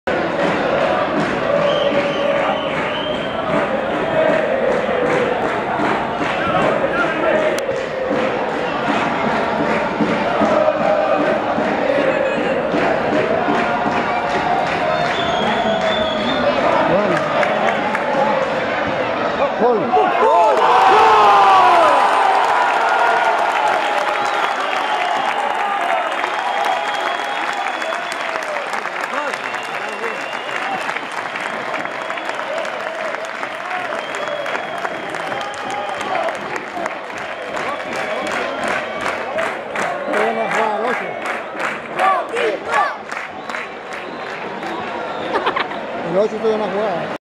Coquimbo Unido, 3er gol a San Felipe, 9 de febrero 2013
gran maniobra ofensiva que termina en el 3er gol de los piratas sobre Union San Felipe